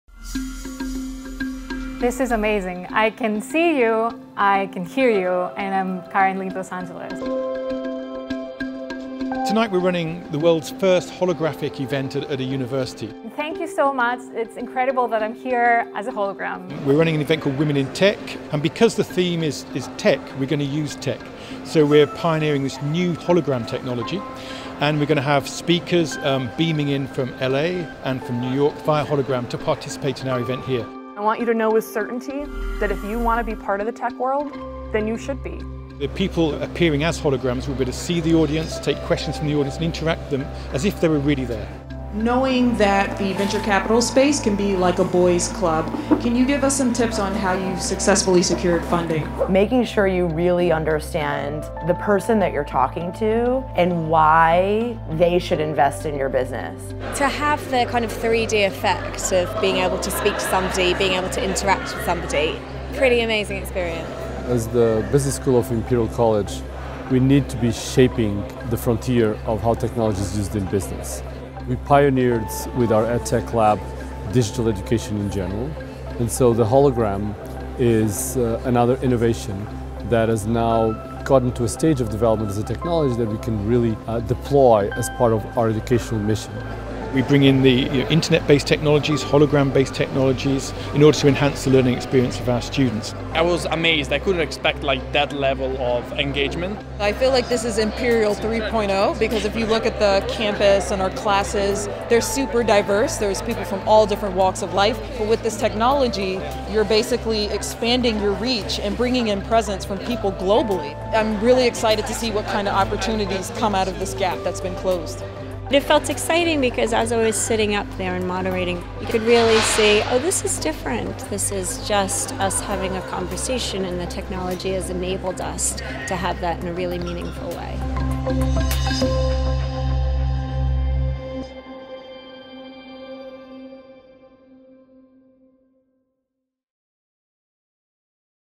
World's first lecture delivered via hologram
University students at Imperial College Business School will be the first in the world to have live lectures delivered to them via hologram.

The technology, developed by Toronto based hologram company AHRT Media and adapted by Imperial College Business School’s pioneering Edtech Lab, will enable faculty and guest speakers to present to students in real time, via a hologram link, from studios located in a variety of global locations including the US, Canada and Singapore.

The hologram will enable lecturers and visiting speakers to appear as 3D, life-size entities within one of Imperial’s lecture theatres. Rather than simply projecting a pre-recorded message, the technology enables those appearing via hologram to engage with their audiences in real-time, responding to audience reactions and taking questions via a camera link in the same manner as if they were physically present in the lecture theatre.

Watch this video to see the hologram in action at the launch event hosted at the Business School.